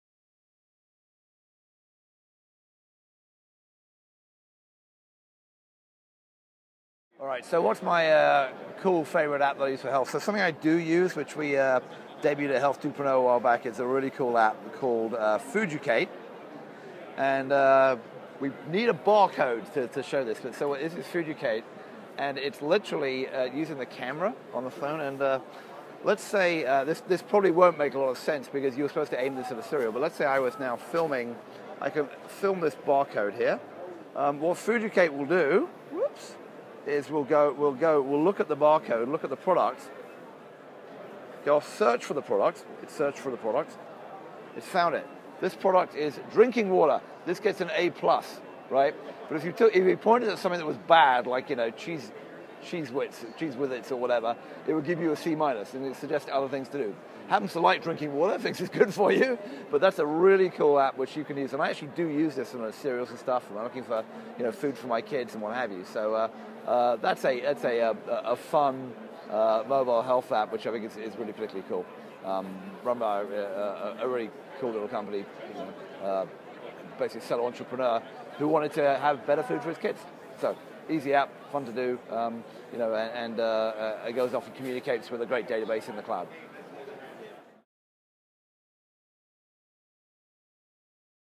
Matthew Holt's Favorite App - HIMSS 2012
At HIMSS12, Matthew Holt stopped by the Kaiser Permanente booth and chatted with the Center for Total Health blog about mobile apps.  Here, Matthew tells us about the one he's loving lately -- Fooducate.